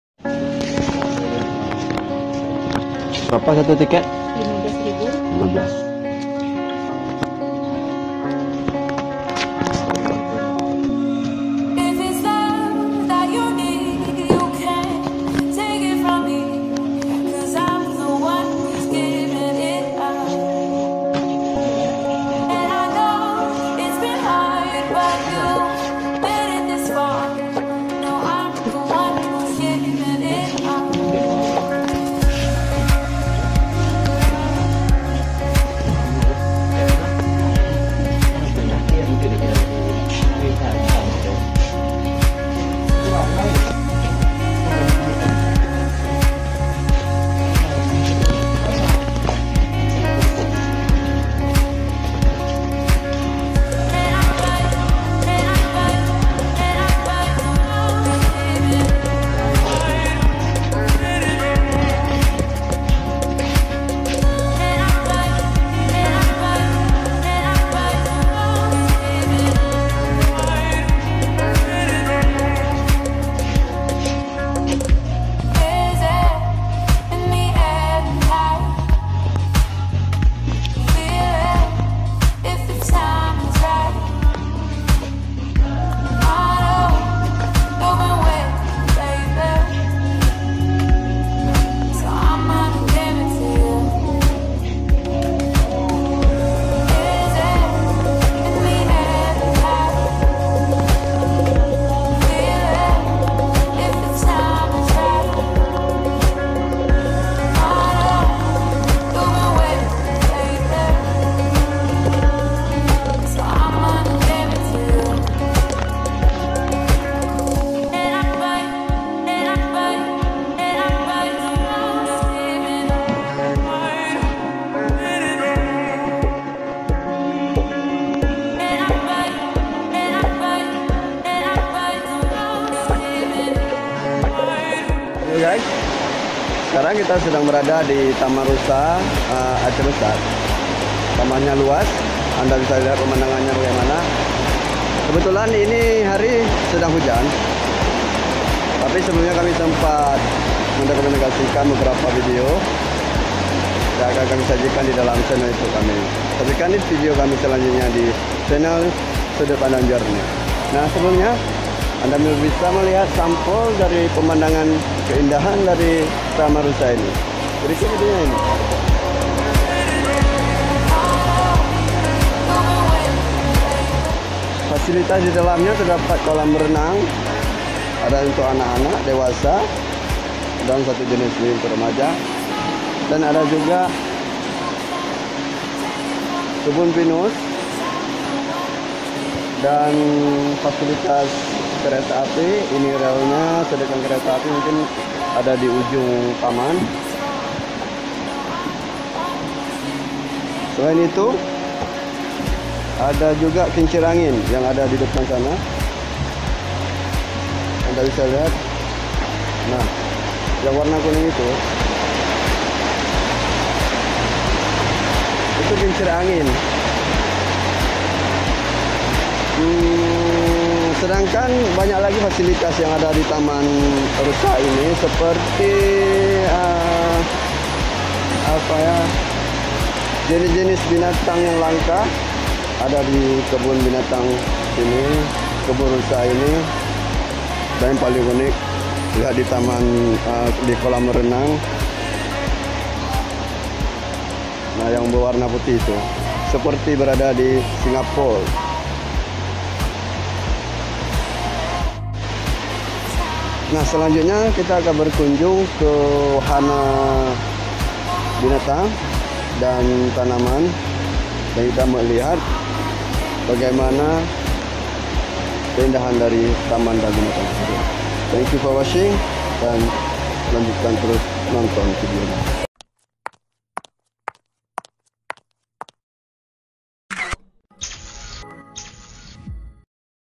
Wisata Taman Rusa Sibreh, Destinasi Tempat Wisata Terlengkap Di Aceh Besar - Travel Vlog
Explore Wisata Taman Rusa, Yang merupakan tempat wisata terlengkap di Aceh, Besar.

• Snack Video: @sudutpandang_77